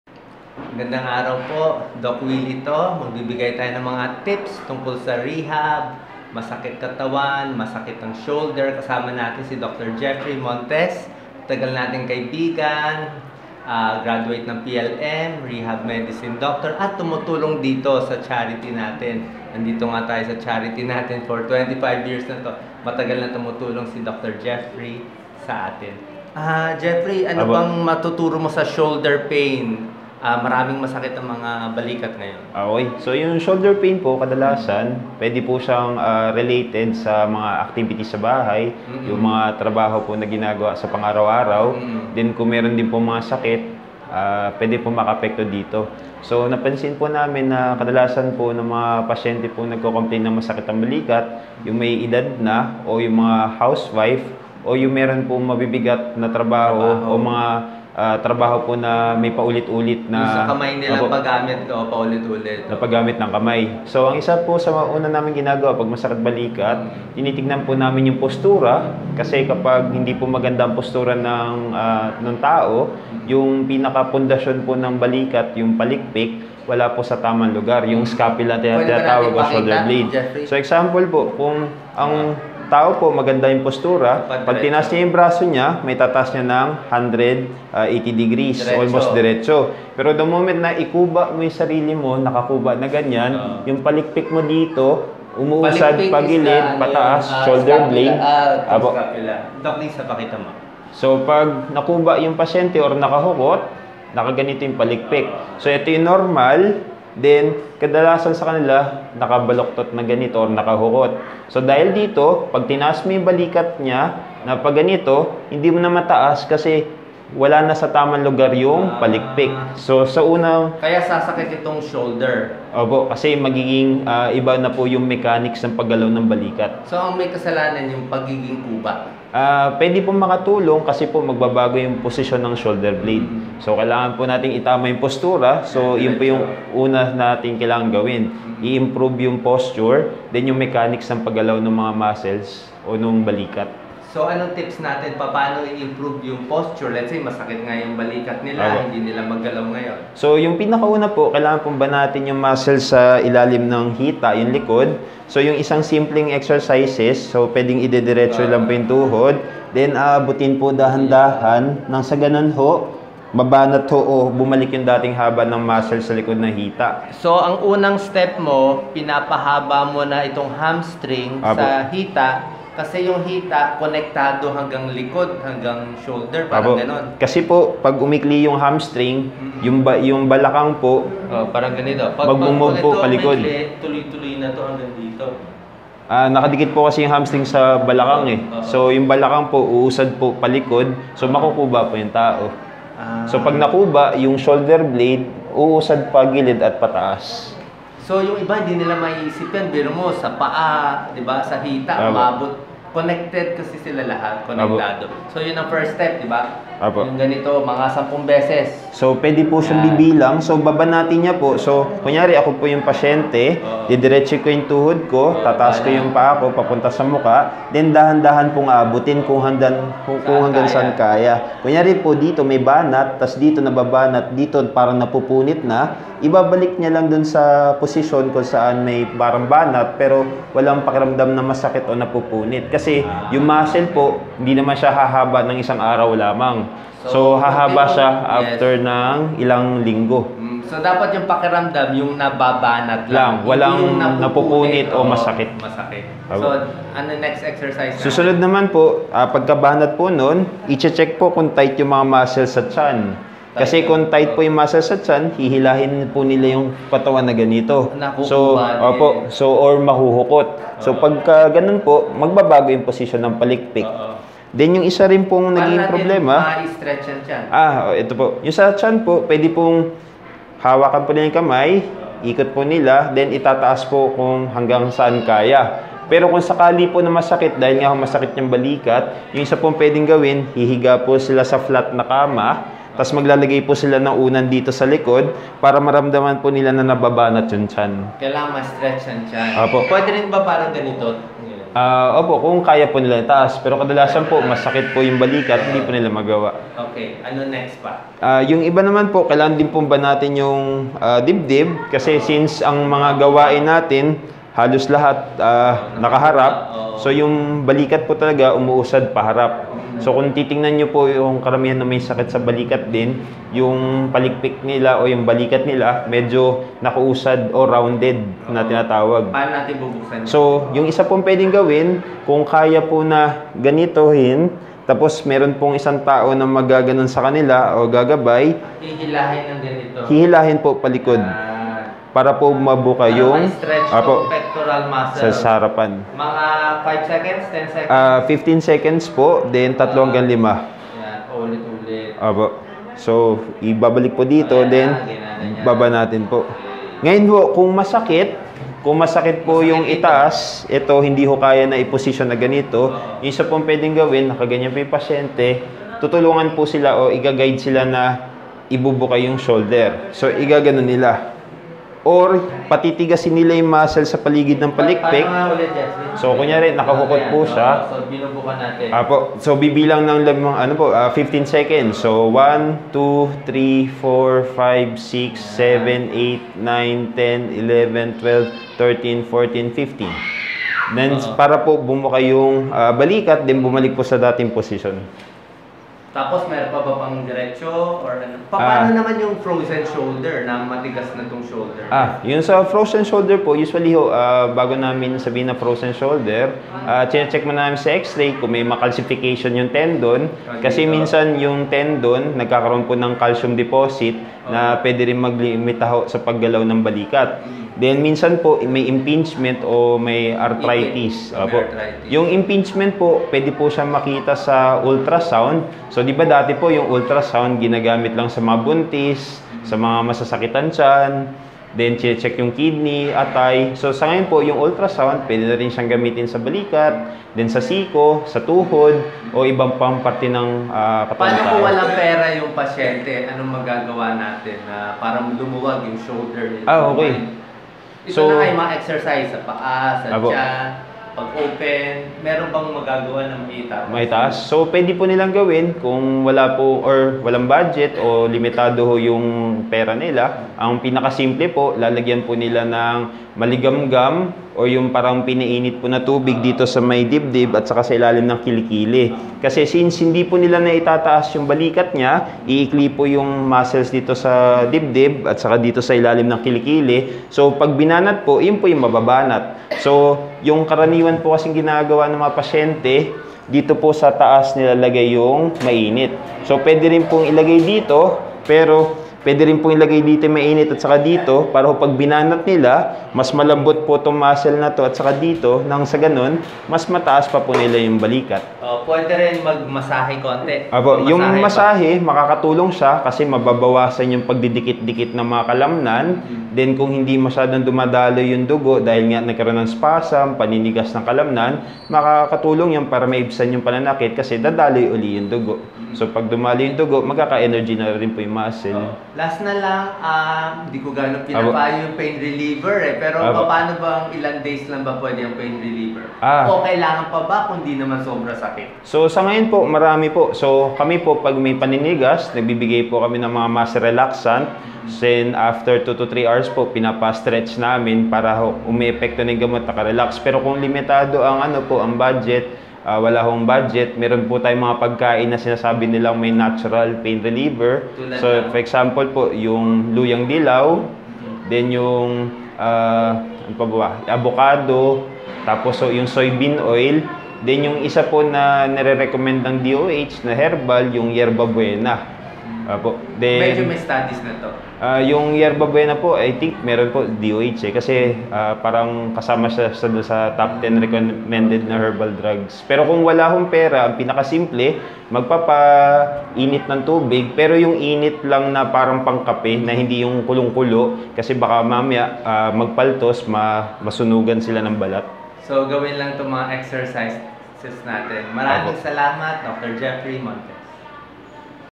Balikat at Likod na Masakit: Frozen Shoulder - ni Doc Jeffrey Montes #1
Balikat at Likod na Masakit (Frozen Shoulder)

Payo ni Doc Jeffrey Montes at Doc Willie Ong #1

1. Heto gagawin sa masakit na balikat o frozen shoulder.
2. I-stretch ang paa, likod at balikat.
3. Lagyan ng hot compress ang balikat at puwede i-masahe.

Si Dr Jeffrey Montes ay isang Rehab Medicine Doctor at matatagpuan sa JBM Rehab Center sa Bulacan, cellphone: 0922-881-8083 at telephone .